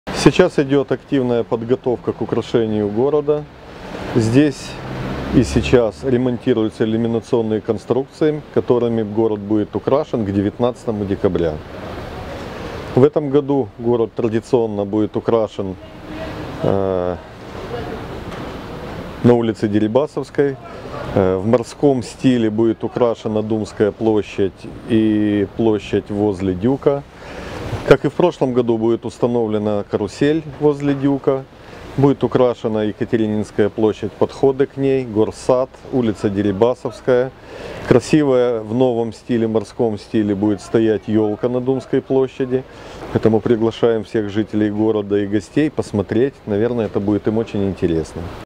Одесу прикрашають до новорічних свят
Коментар директора КП «Одесміськсвітло» Олександра Прокопця